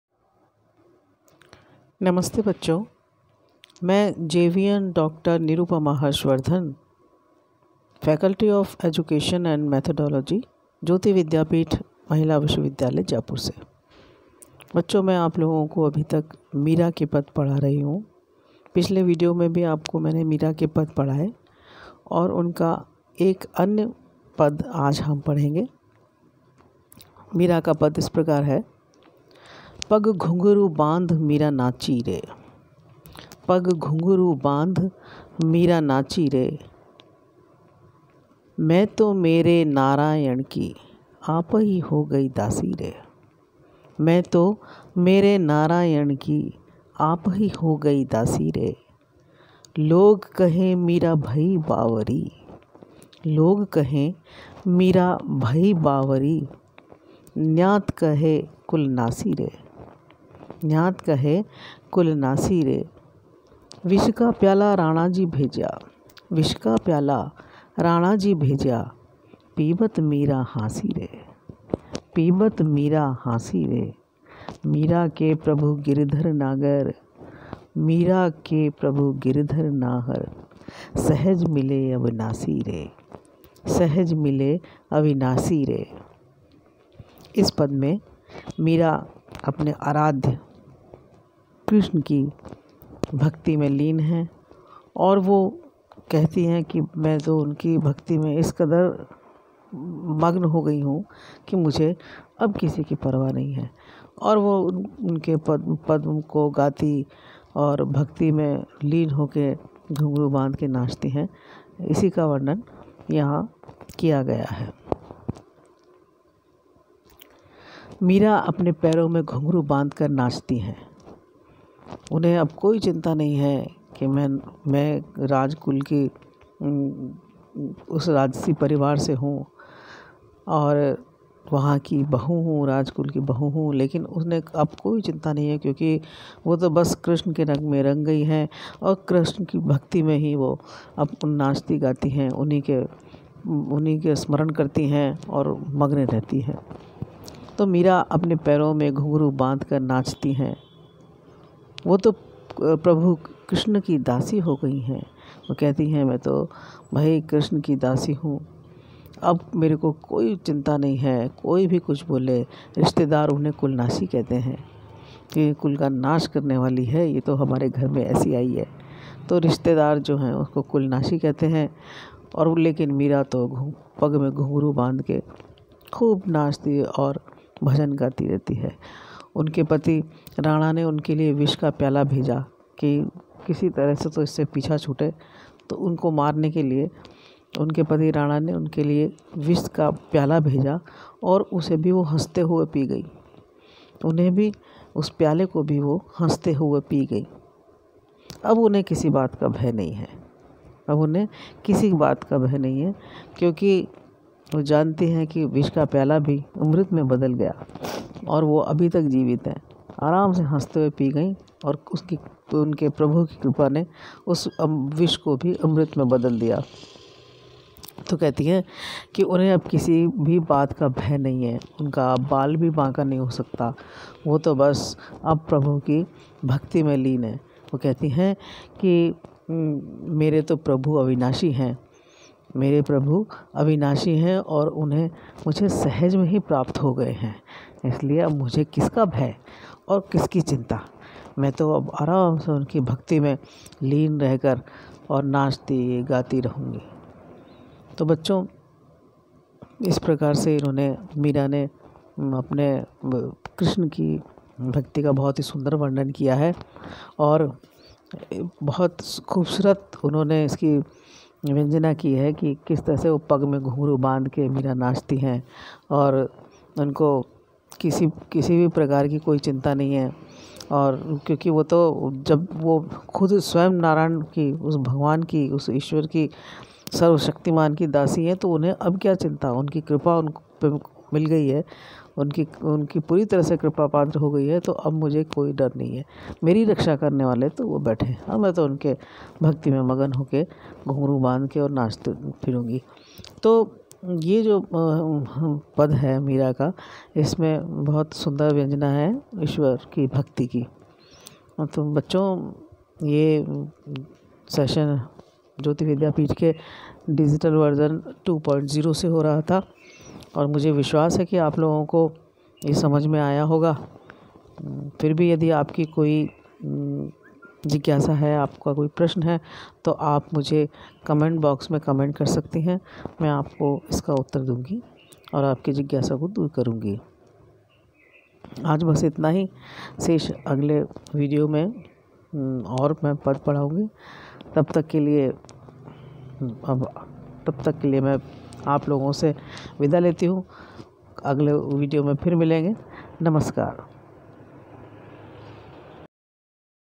JAYOTI VIDYAPEETH
JAYOTI VIDYAPEETH WOMEN UNIVERSITY JAIPUR
FACULTY OF EDUCATION
FACULTY NAME DR NIRUPAMA
TOPIC NAME BA BEd II sem Hindi language Meera ke pad